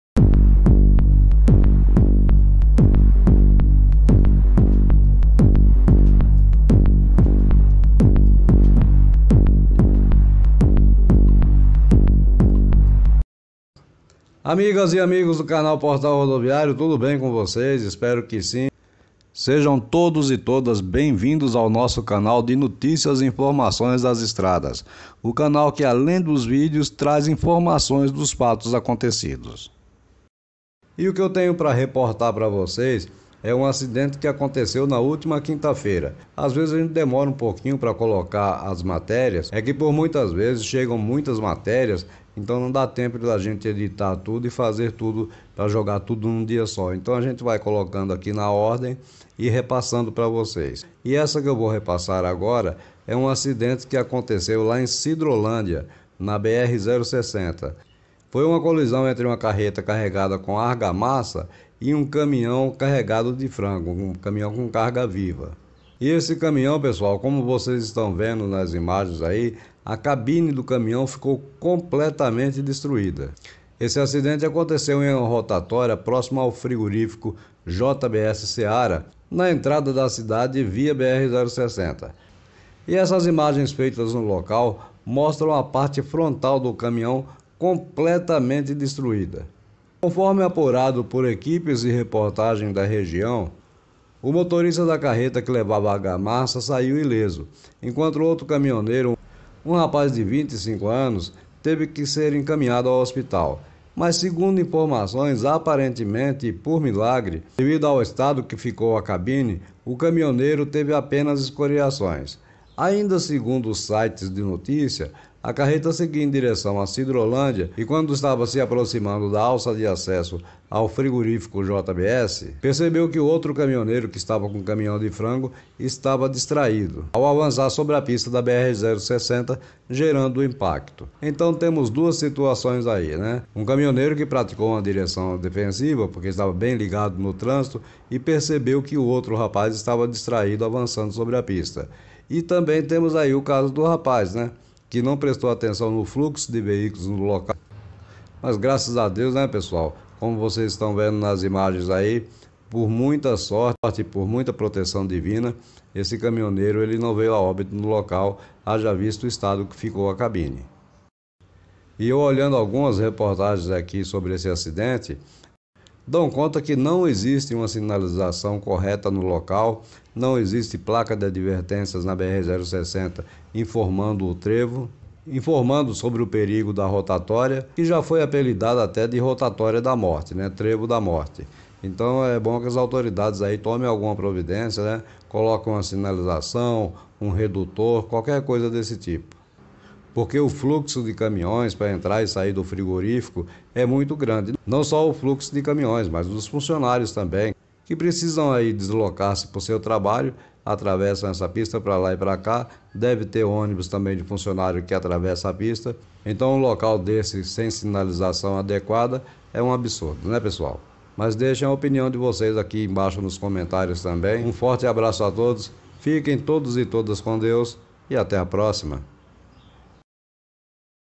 Caminhão não para em rotatória e carreta arranca cabine na BR 060
Colisão entre uma carreta e um caminhão deixou cabine de um dos veículos praticamente toda destruída na manhã desta quinta-feira (6) em Sidrolândia, a cerca de 74 quilômetros de Campo Grande.
O acidente aconteceu em rotatória da próximo a frigorífico da JBS/Seara, na entrada da cidade via rodovia BR-060.
 CREDITO: CAMPO GRANDE NEWS e Circolinotv
Siga nossa Página no Fecebook
Link abaixo